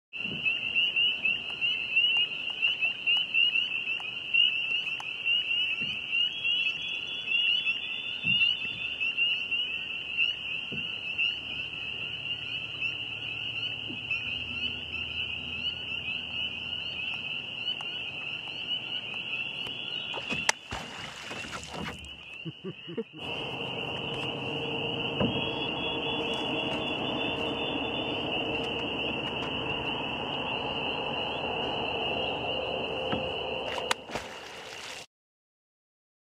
Beaver Tails Slapping on James Lake
Another canoe ride, another wildlife sighting; this time, a family of beavers.